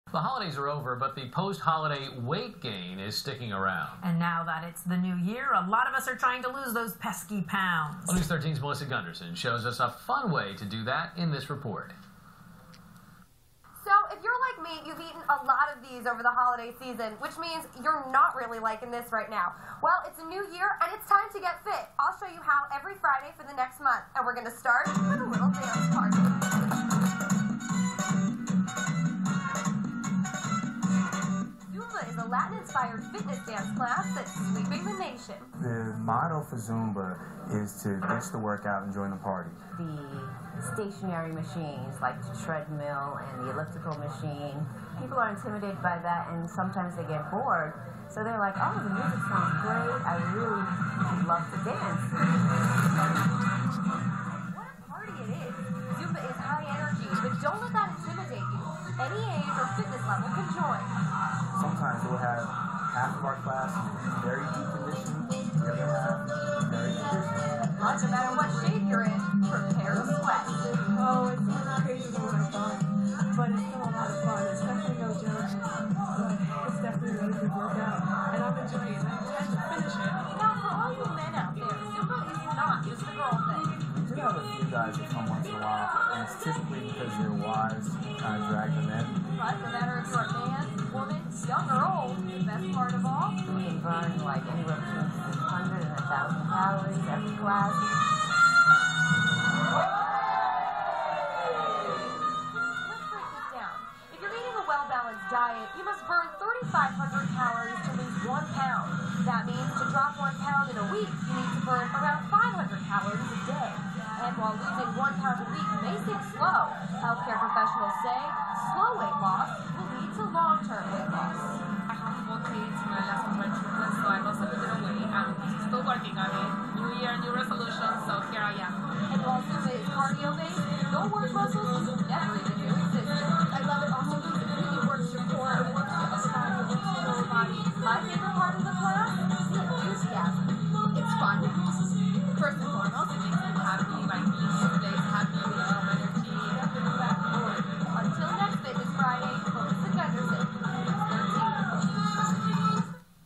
Elevations TV-13 Fitness Friday Zumba Jan 2012
Elevations hosted TV-13 reporter Melissa Gundersen for her report on Zumba.